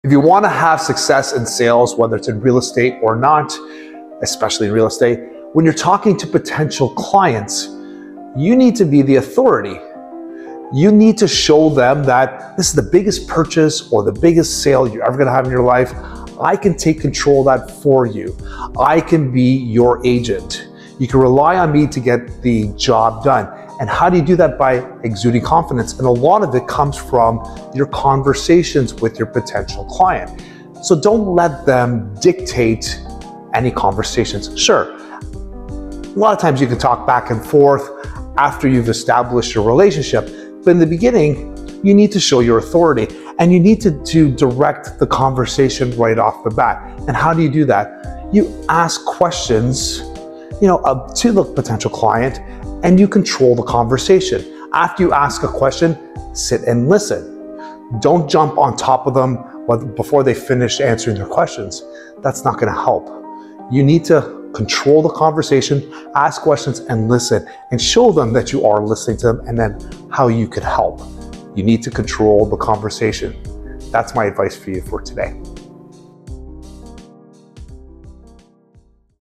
This is how to be successful in sales
Friday Real Estate Advice💡- This is how to be successful in sales

Success in sales starts with mastering the art of conversation. 🎤💼 Be the authority, lead the dialogue, and truly listen to your clients' needs. It's not just about speaking—it's about understanding and responding with expertise. 🌟🗣️